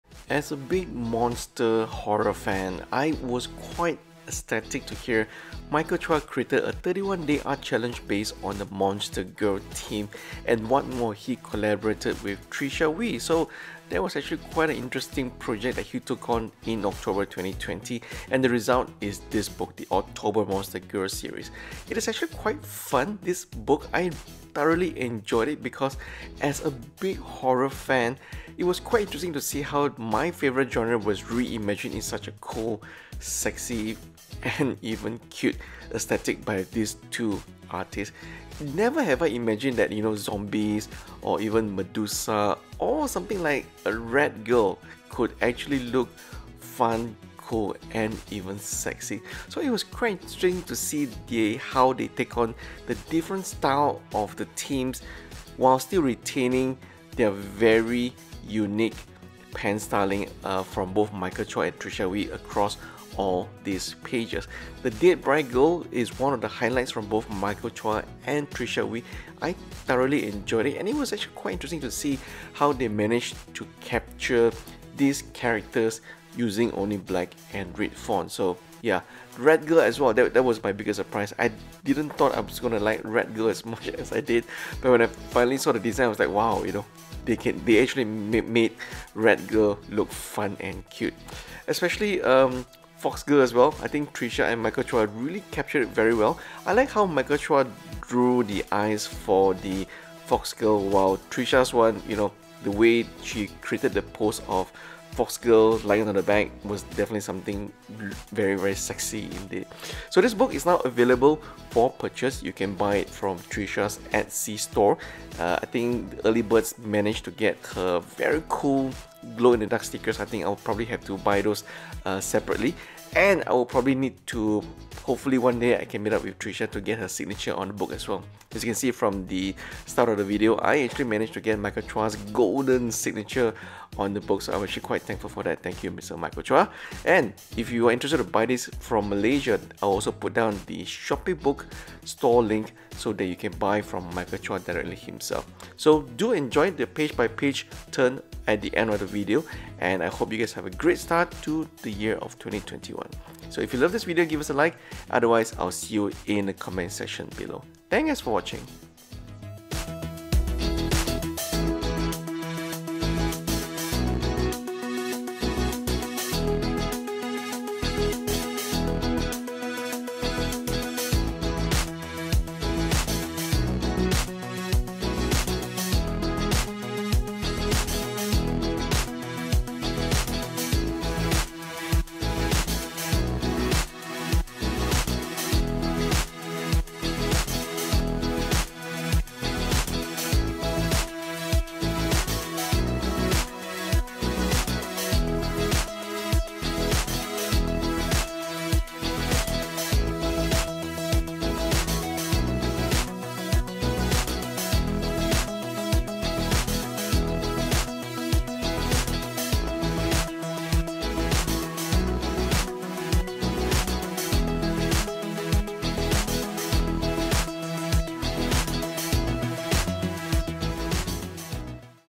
Oddtober Monster Girl Book Review! Page by Page Reveal!
A very cool collaboration Art book by Michael Chuah and Tricia Wee on my favourite genre : Monsters!!